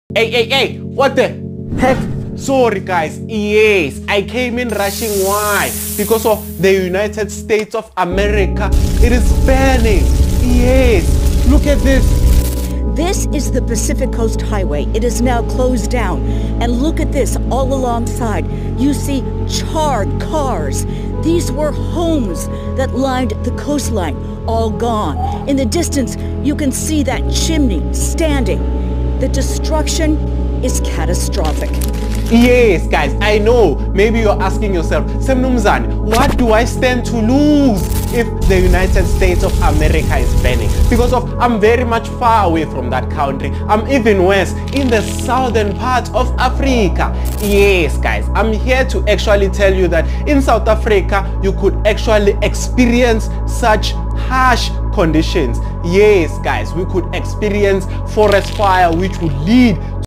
Berg Winds iYessss AsiyEen'✍️🏾😁🔥✊🏾 southafrica geography matric #2025
What Are Berg Winds? A Detailed Explanation and Weather Implications

In this video, we dive deep into the fascinating phenomenon of Berg Winds, a unique weather pattern that occurs in certain regions, particularly in South Africa. But what exactly are Berg Winds, and why are they so impactful?

We’ll explore:

The science behind Berg Winds, including how they form and the atmospheric conditions that lead to these strong, dry winds.

The effects of Berg Winds on local weather, including temperature shifts, droughts, and fire risks.

How Berg Winds influence environmental conditions and the local climate, especially in mountainous and coastal areas.

Real-life examples of Berg Winds in action, especially in South Africa, where they play a key role in shaping the weather.

Whether you’re a weather enthusiast or just curious about this intriguing natural phenomenon, this video will give you a clear and comprehensive understanding of Berg Winds.